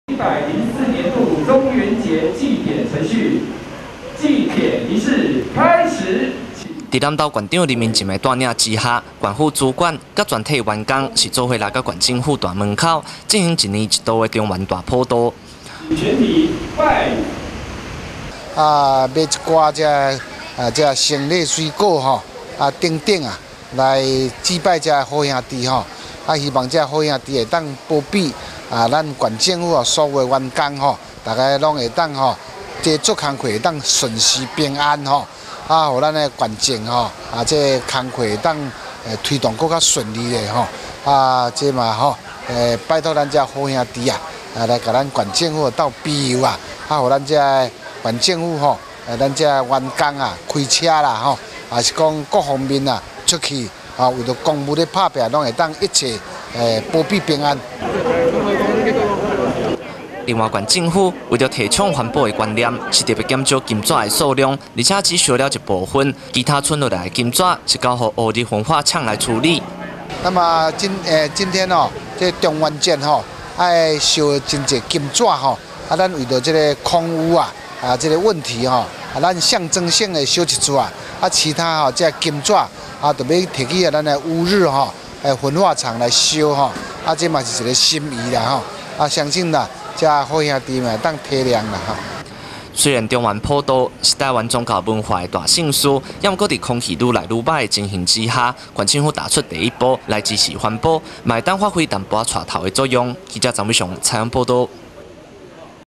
南投新聞-縣府普渡帶頭防空污
農曆七月十五日中元節，南投縣政府提早一天來普渡，在南投縣長林明溱的帶領之下，縣政府主管與全體員工，完成一年一度的祭拜儀式，另外縣府為了提倡環保的觀念，特別減少金紙的數量，並且只有象徵性的燒一部分，剩下來的金紙，將送去烏日焚化廠來處理，中元節雖然是宗教習俗不可馬虎的大祭典，不過縣長林明溱則是笑說：「相信好兄弟能夠體諒。」